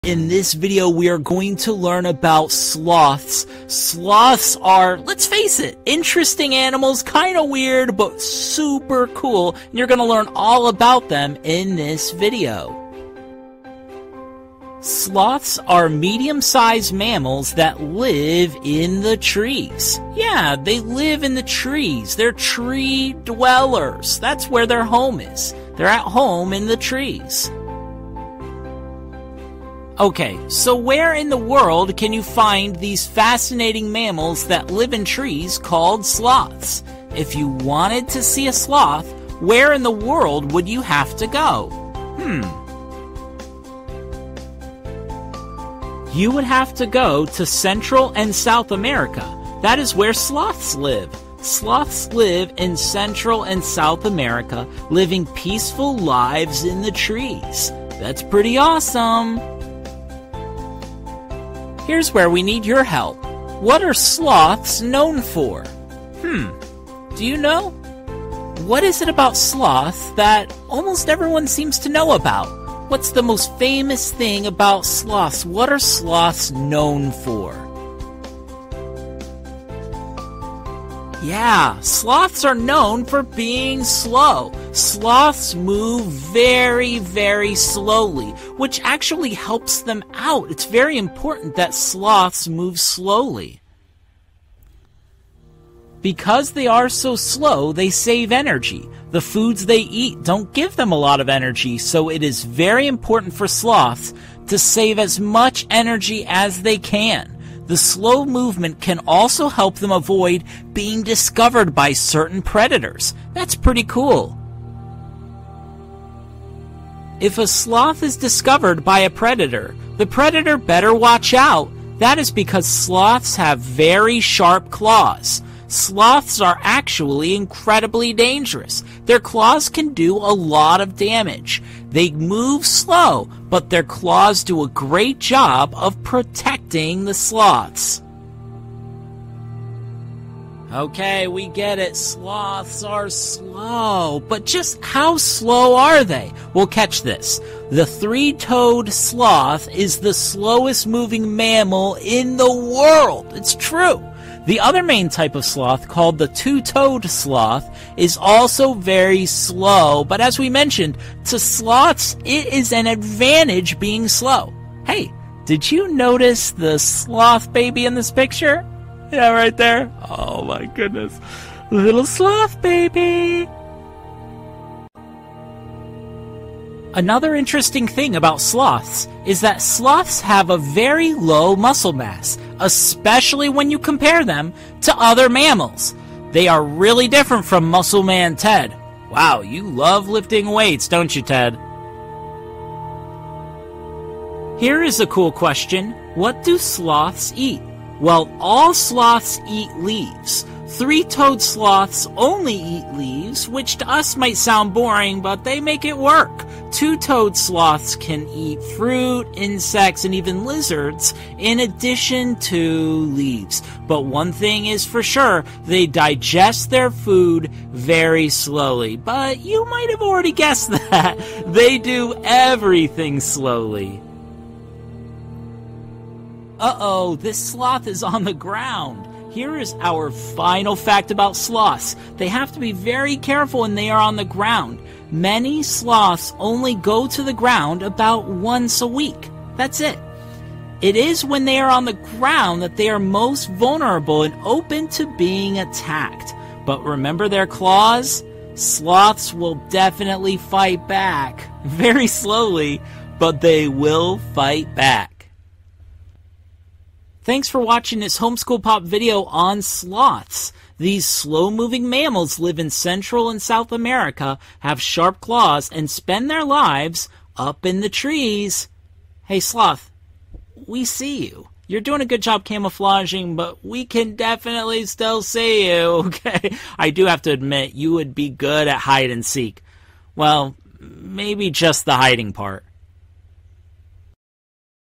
Sloths for Kids
Sloths! In this sloth video for kids, learn all about these amazing mammals that live in the trees of Central and South America! See just how slow sloths are, and some other cool facts as well!